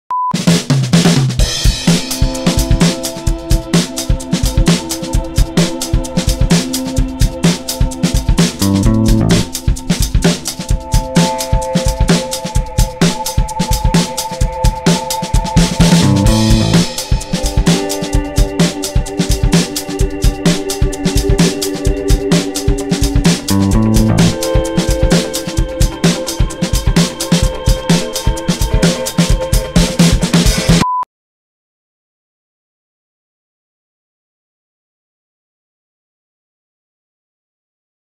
Vinheta Coletivo
Vinheta de Abertura da Campanha eufaçoamostra para a 18ª Mostra de Cinema de Tiradentes